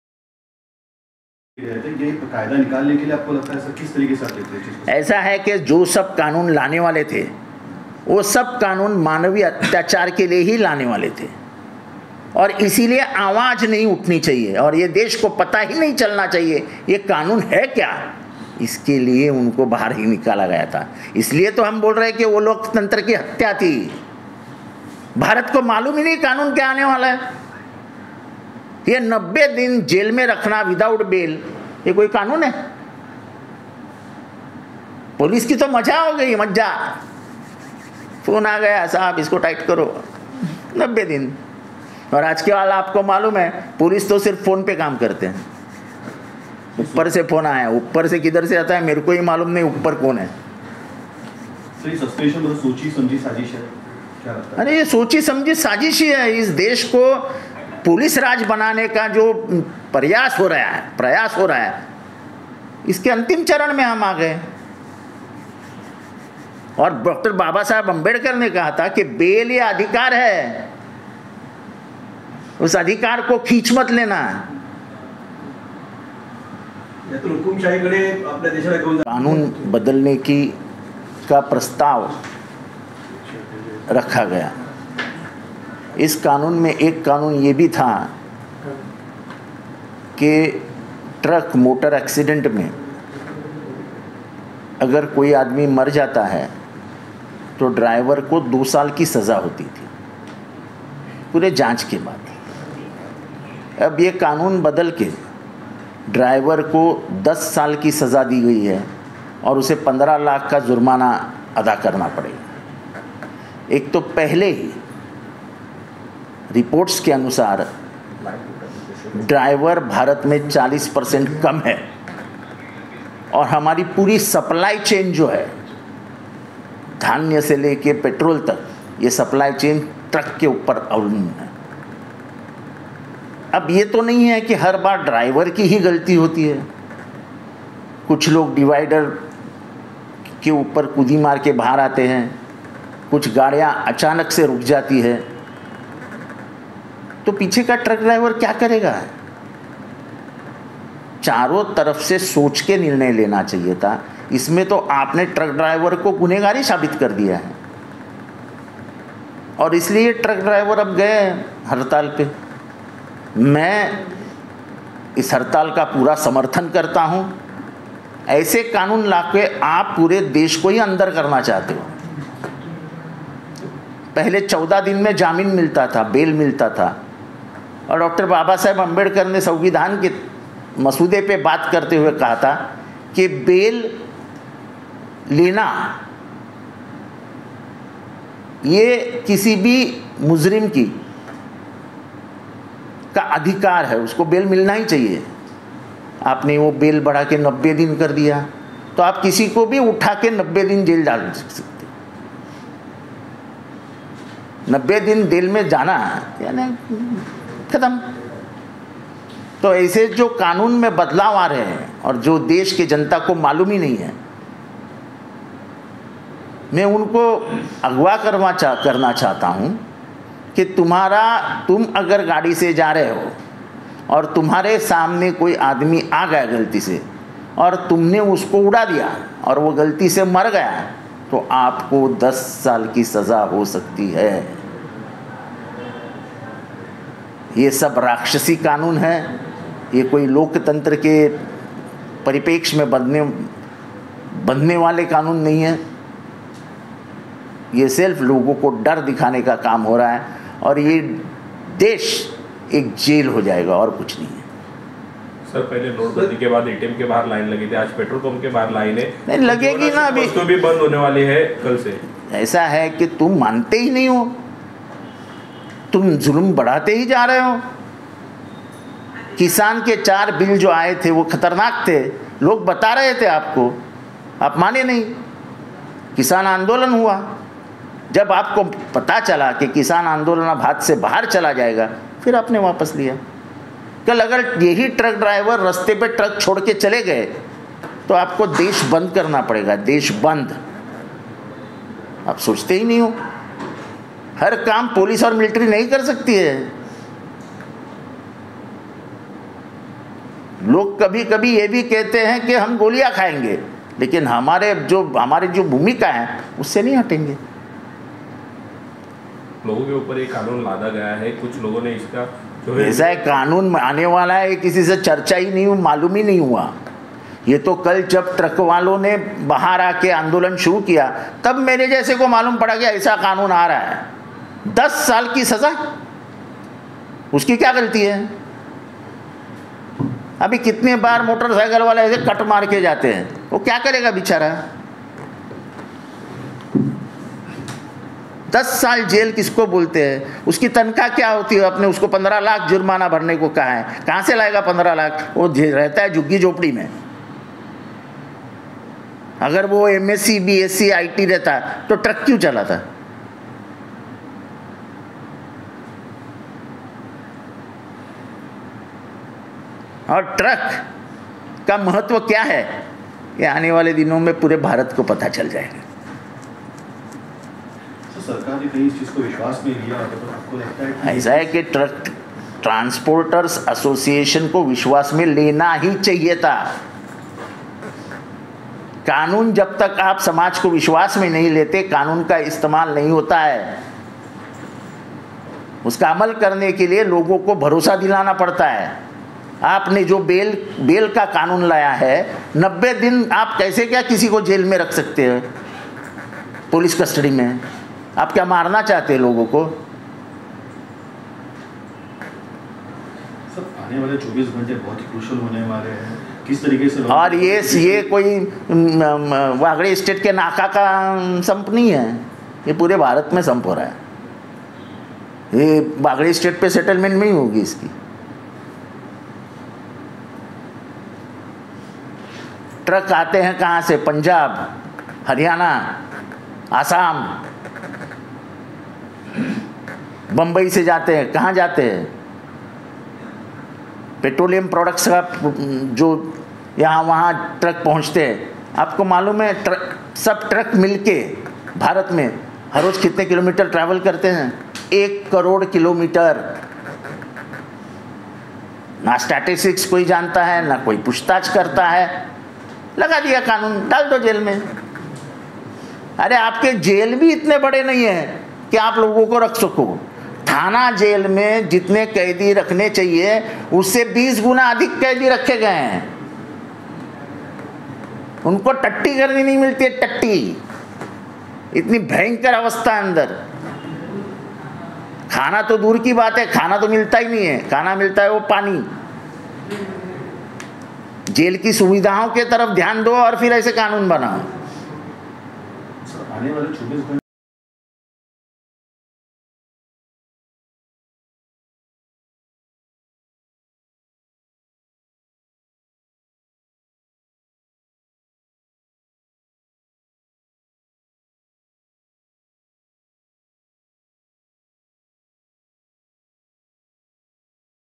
जितेंद्र आव्हाड का हल्ला बोल केंद्र सरकार ने राक्षसी कानून पारित किया || Islamuddin Khan
केंद्र सरकार ने राक्षसी कानून पारित किया है... जितेंद्र आव्हाड का केंद्र सरकार पर हल्लाबोल.

इस देश को पुलिस राज बनाने के अंतिम चरण पर हम आ गए हैं, पुलिस की तो मजा ही मजा हो गई है, पुलीस अब सिर्फ फ़ोन पर ही काम करेगी...

यह डॉक्टर बाबा साहब आंबेडकर ने कहा की अधिकार है इसे खेच मत लेना लेकीन आज भाजपा वही कर रही है.

कुछ दिनों पहले सदन में ये कई प्रस्ताव रखे गए थे,जिनमे एक प्रस्ताव मोटर वाहनों को लेकर भी था...

पहले कानून था की अगर ड्राइवर के जरिए एक्सीडेंट में किसी की मौत होती है,तो उसे दो साल की सजा होती थी,और वो भी जांच करने के बाद।लेकिन वर्तमान मोदी सरकार ने इस कानून को बदलकर अब ड्राइवर की सजा को 10 साल कर दिया है,और 15 लाख रुपए जुर्माना रखा गया है ।

आव्हाड ने कहा है,एक तो पहले से देश में ड्राइवरों की संख्या गाड़ियों की मुकाबले 40 प्रतिशत कम है,और देश की जो पुरी सप्लाई चैन जो है,वो इन ड्राइवरों के जरिए ही देश के अलग अलग  राज्यों में खाने पीने की चीज से लेकर पेट्रोल तक देश में पहुंचाए जाते है।

आव्हाड ने कहा है,की सड़क पर कुछ लोग ऐसे भी होते है,जो डिवाइडर को लांघ कर जाते है।कुछ गाड़ियों की रफ्तार किसी कारण वश अचानक से कम हो जाती है,या रूक जाती है,ऐसे में पीछे आ रहा ट्रक ड्राइवर क्या करेगा,सरकार ने तो सीधे सीधे ट्रक ड्राइवर को ही अपराधी बना दिया है।इसलिए ट्रक ड्राइवर हड़ताल पर गए है,और मैं इस हड़ताल का पुरा समर्थन करता हूं।केंद्र सरकार के इस कानून को जितेंद्र आव्हाड ने राक्षसी कानून बताया है।

बाइट - जितेंद्र आव्हाड (इंडिया गठबंधन राष्ट्रीय प्रवक्ता, विधायक और नेता शरद पवार एनसीपी)

आव्हाड के मुताबिक इस तरह का कोई कानून आनेवाला है,या लागू होनेवाला है,देश में किसी को पता ही नही था,ये तो जब ट्रक वालो ने कल से हड़ताल करना शुरू किया,तब सभी को। पता चला।10 साल की सजा का प्रावधान क्या होता है।ट्रक ड्राइवर की सेलरी कितनी होती है,वो 15 लाख रुपए कहा से लेकर आएगा,वो रहता है,झुग्गी -  झोपड़ी में ।और ट्रक और ड्राइवर का महत्व क्या होता है,ये आनेवाले दिनों में पुरे भारत को पता लग जाएगा।सरकार ने ट्रास्पोटेर्स एसोसियेशन को भी विश्वास में नही लिया,उनसे बिना चर्चा कर जबरन ये कानून देश पर लाद दिया।जब कोई कानून पारित होता है,तो लोगो को समाज को विश्वास में लाया जाता है,90 दिन तक कैसे आप।किसी को जेल में रखा सकते हो।ये हड़ताल कोई वागले स्टेट में नही हो रहा है,की सेटिंग होगा,ये हड़ताल पुरे भारत में हो  रहा है।

Byte : जितेंद्र आव्हाड (इंडिया गठबंधन राष्ट्रीय प्रवक्ता, विधायक और नेता शरद पवार एनसीपी)Aapka Prahar Times